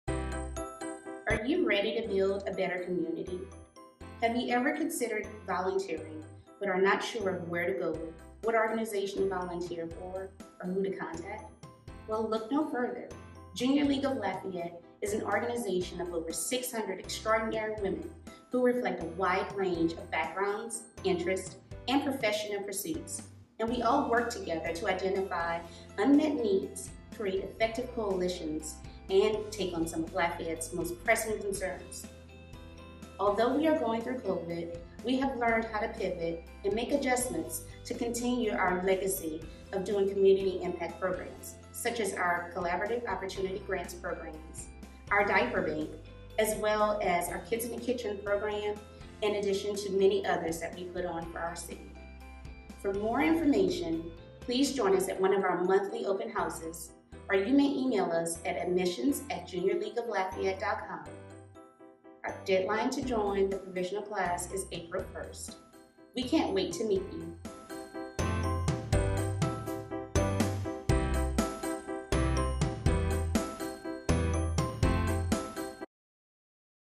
Junior League of Lafayette Admissions Open House
Join Adoria Hankton, Junior League of Lafayette's Admissions Chair, for one of our monthly Open Houses to learn more about League membership.